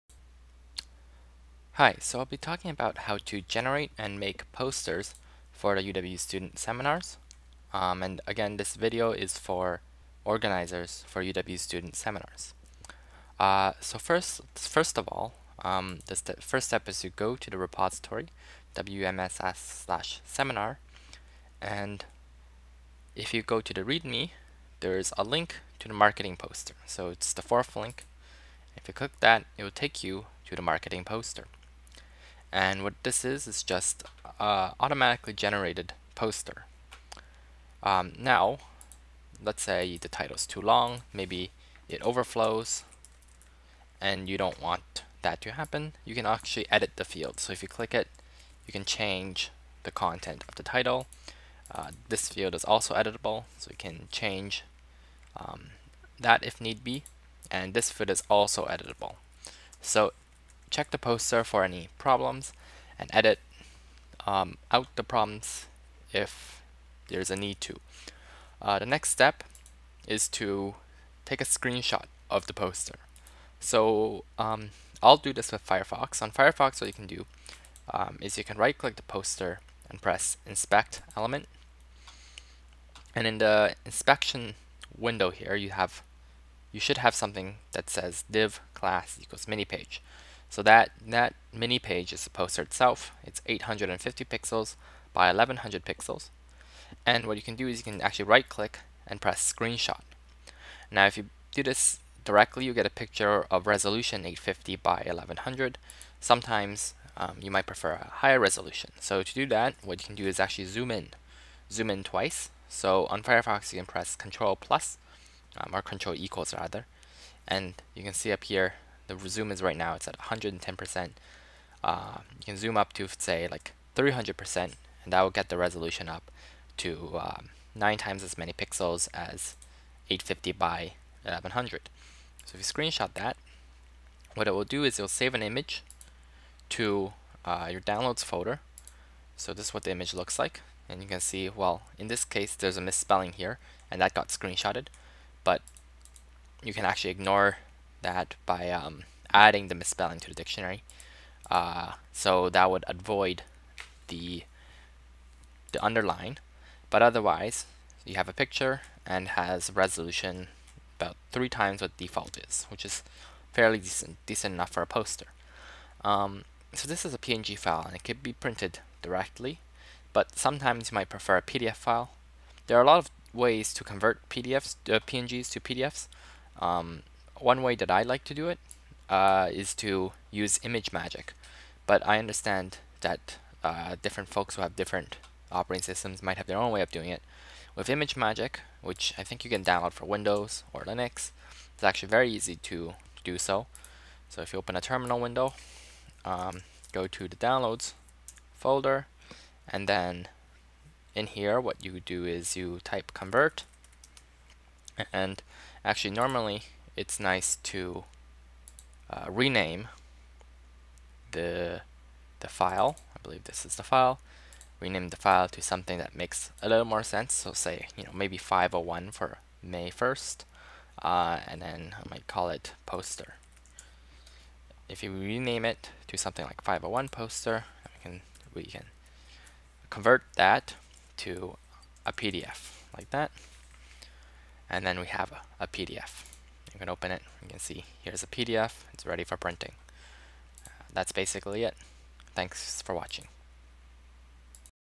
Student Seminars Website: Making Posters
In this video I go over how to screenshot posters.